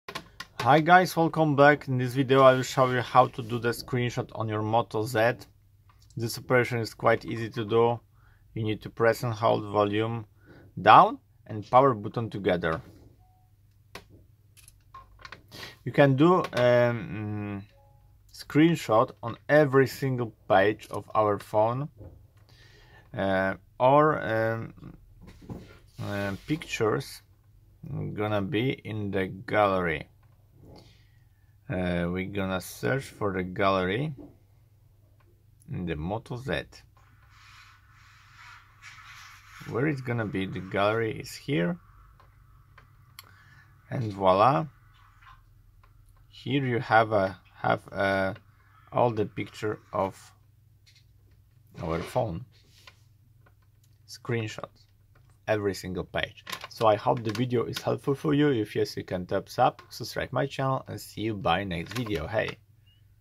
How to: Lenovo Moto Z Screenshot
In this video, we show you how to take and share a screenshot with the Moto Z.

► it was helpful? LIKE IT :-) THX